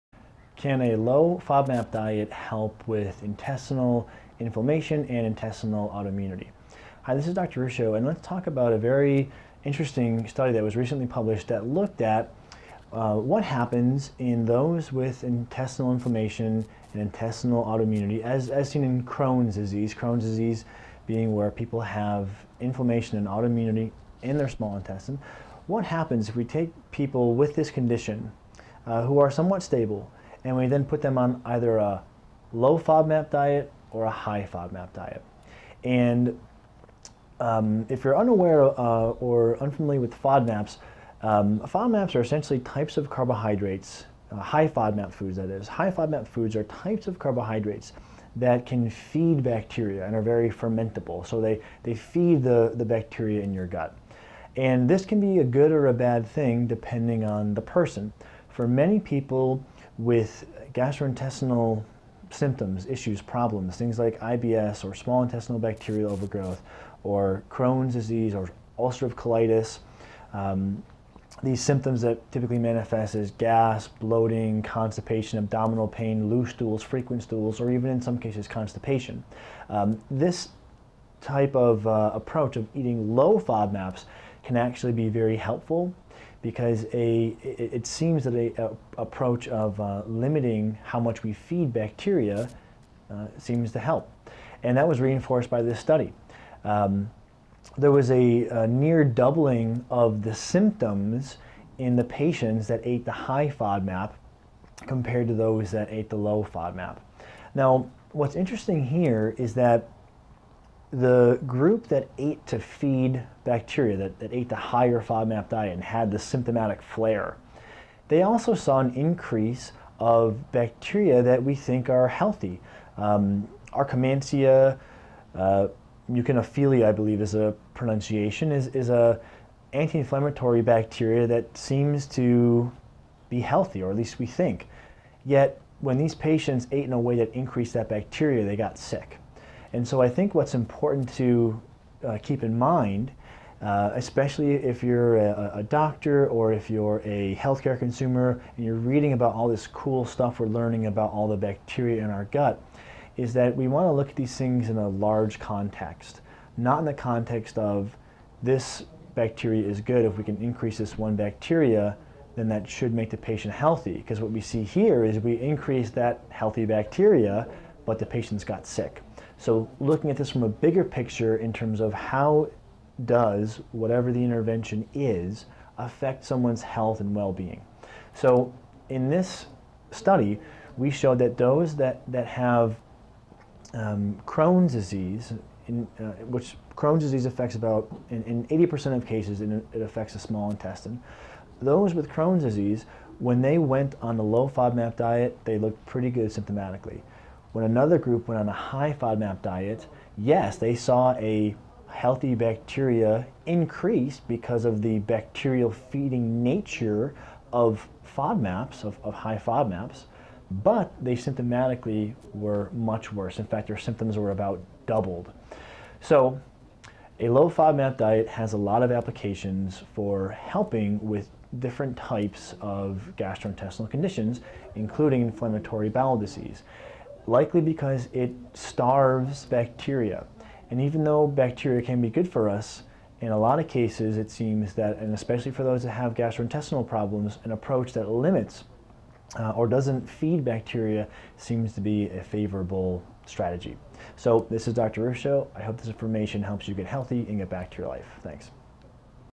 Low-FODMAP Diet for Intestinal Inflammation and Autoimmunity
Can a low-FODMAP diet help with small intestinal inflammation and autoimmunity? Let’s discuss a recent study where two groups were fed either a high- or a low-FODMAP diet, expand on what happened to gut bacteria, and more importantly the effect on symptoms. Want more info?